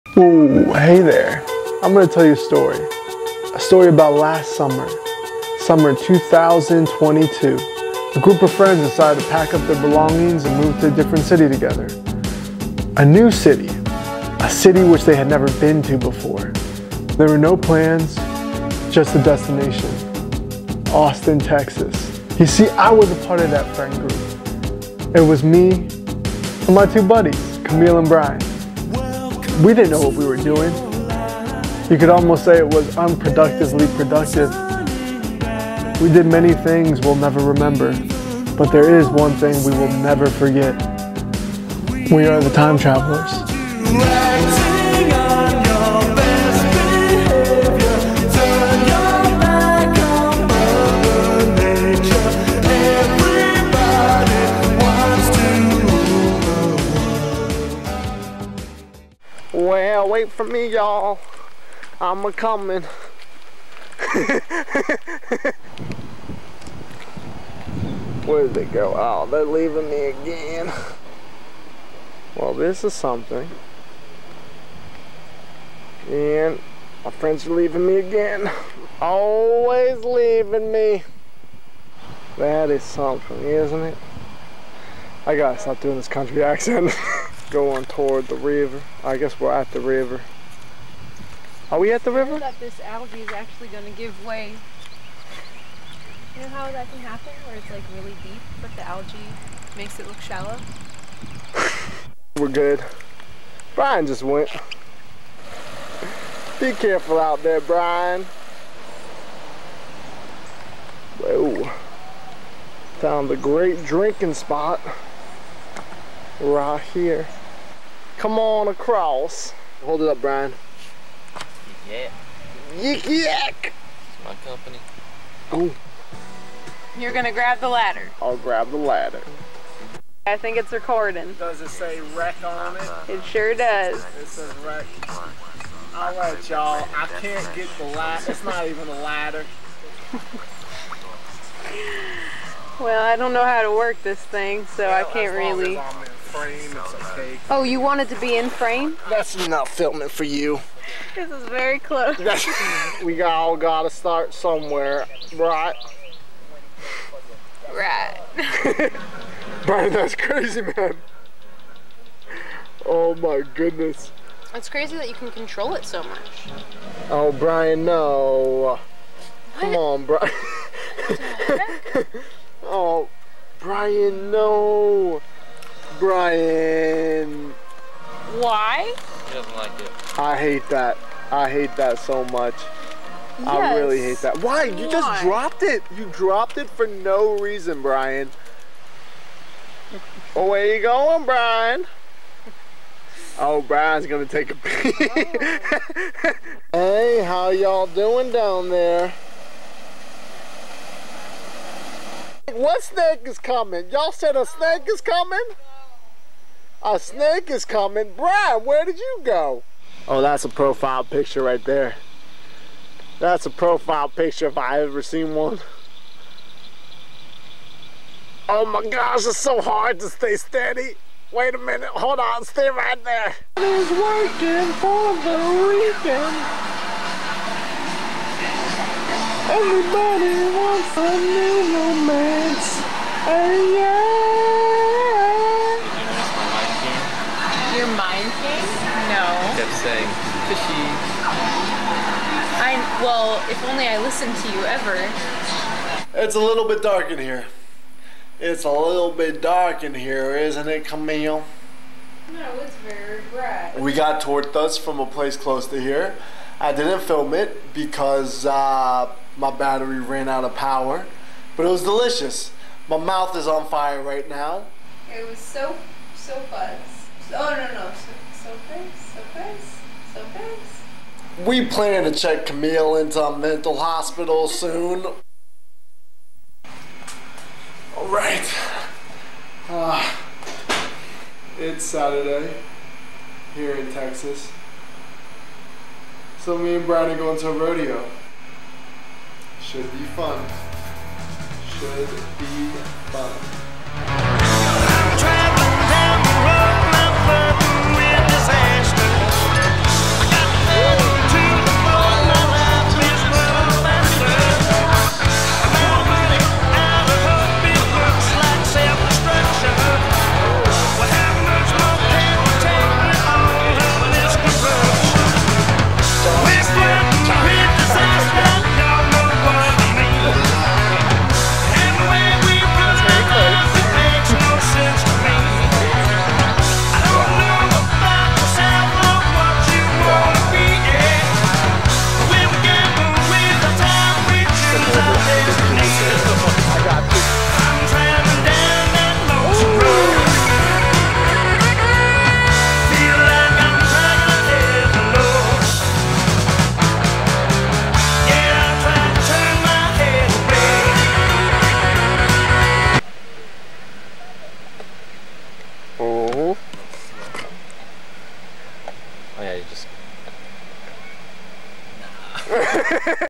VHS-C Tapes: Flirtin' With Disaster (Time Travelers Ep. 4)
Have y'all ever been to a rodeo?

Just some other old-old VHS-C tapes I found in my bedroom.

Filmed on a JVC GRC7U VHS camera!